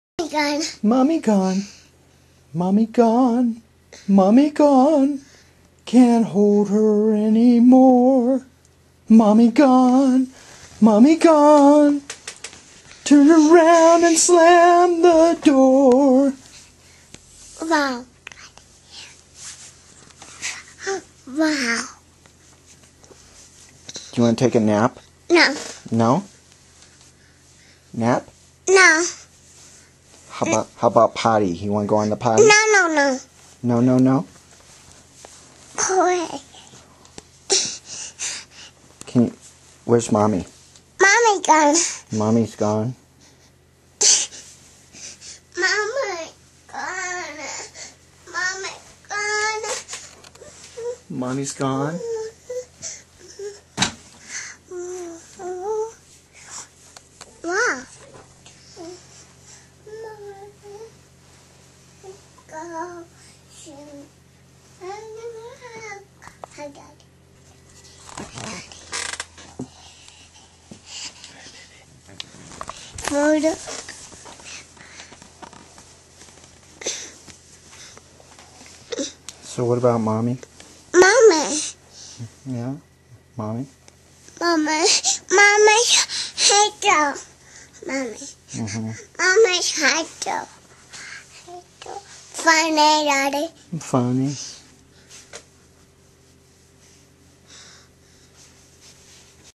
Zoe Missing Mommy
Zoe is in Daddy's office and missing Mommy on October 2, 2014.